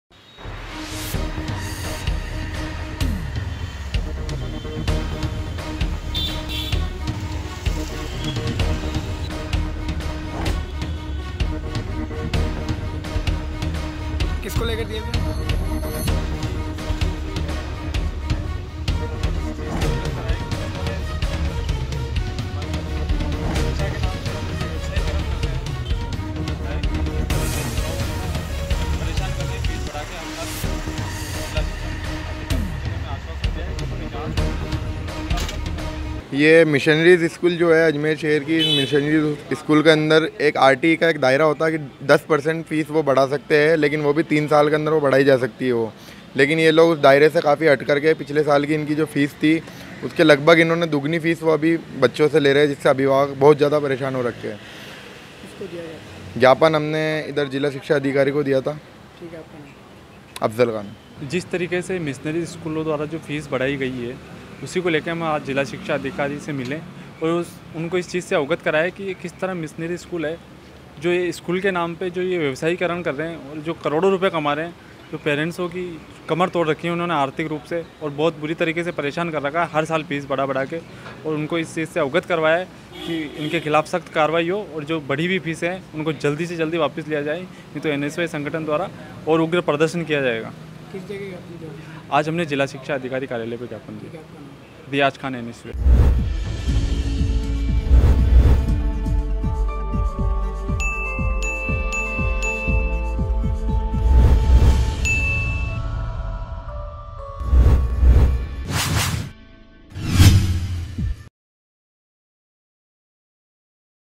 AJMER NEWS | मिशनरी स्कूल के खिलाफ NSUI का प्रदर्शन जारी | MTTV INDIA
मिशनरी स्कूल के खिलाफ NSUI का प्रदर्शन जारी
NSUI के प्रतिनिधिमंडल ने जिला शिक्षा अधिकारी को सौंपा ज्ञापन
जिला शिक्षा अधिकारी को मिशनरी स्कूल में बड़ी फीस की दी जानकारी
NSUI का आरोप मिशनरी स्कूल द्वारा अवांछित शुल्क वृद्धि